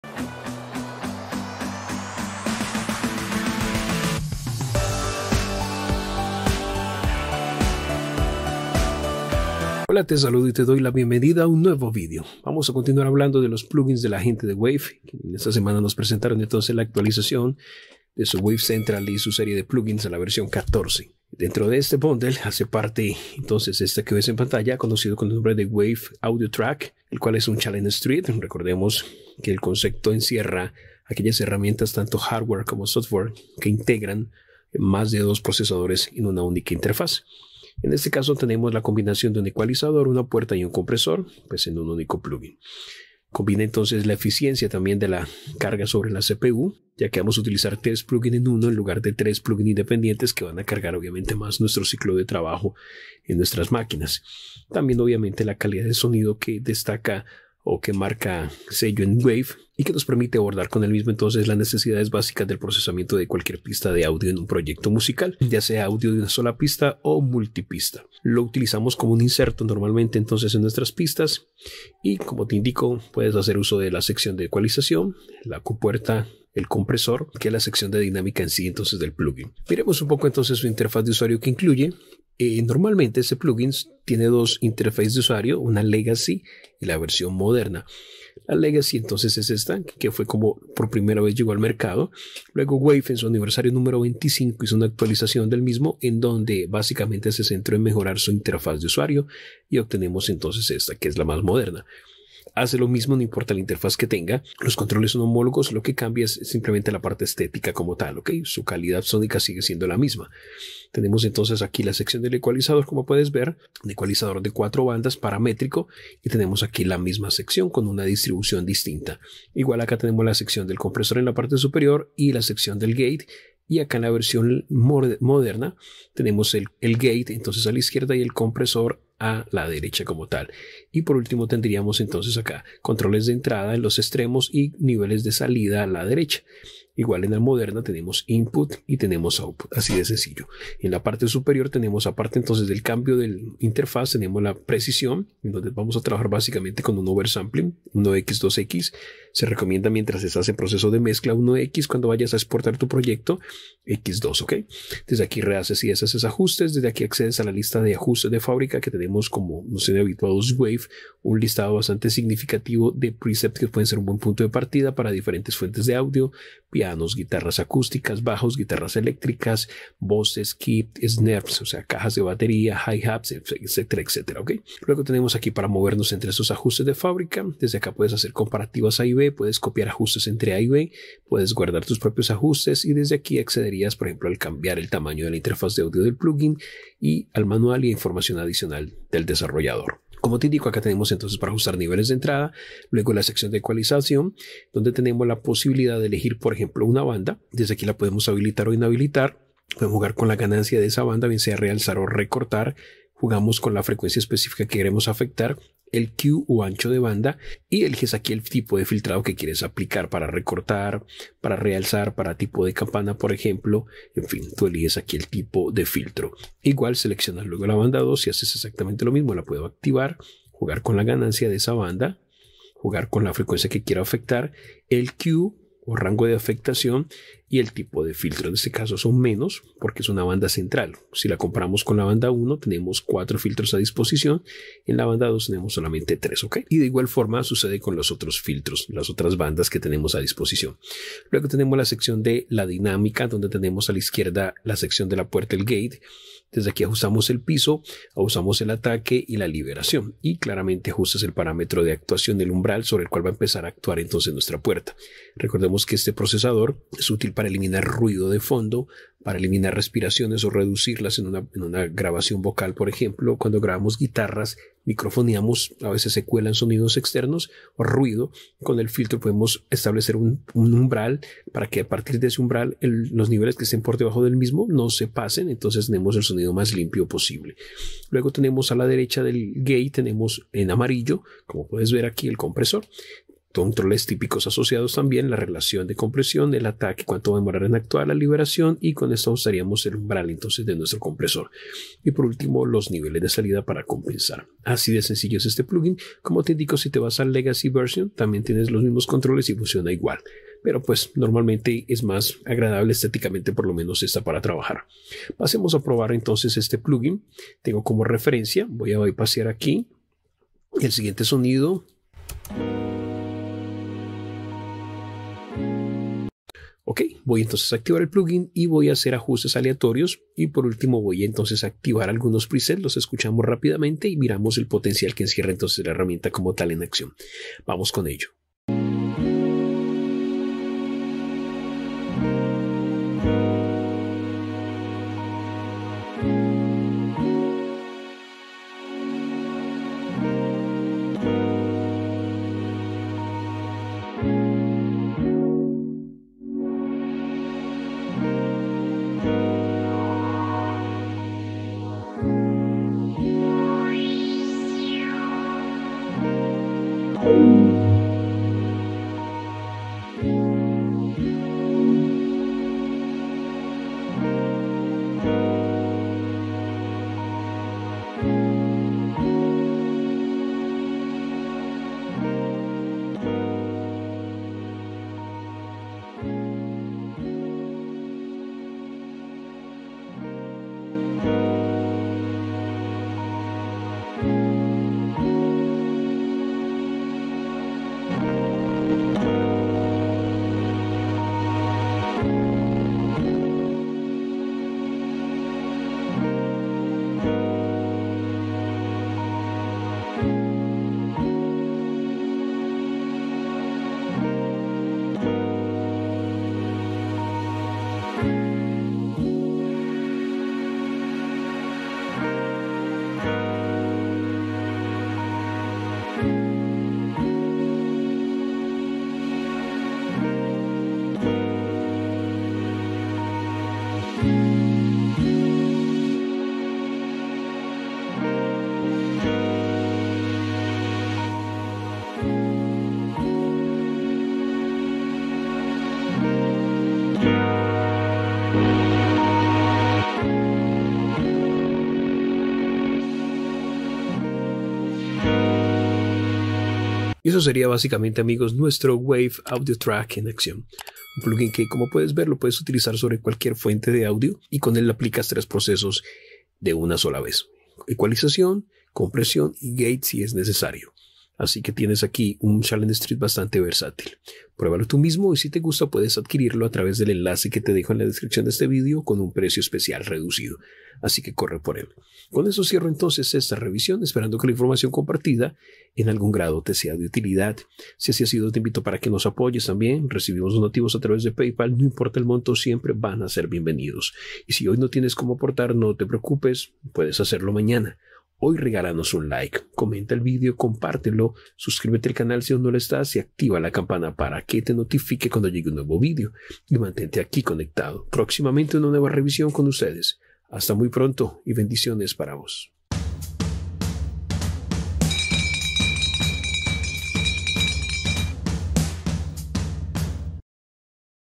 ⚡Waves AudioTrack⚡ -Revisión en Español-
Un sencillo y versátil Channelstrip con la calidad de Waves detrás del mismo.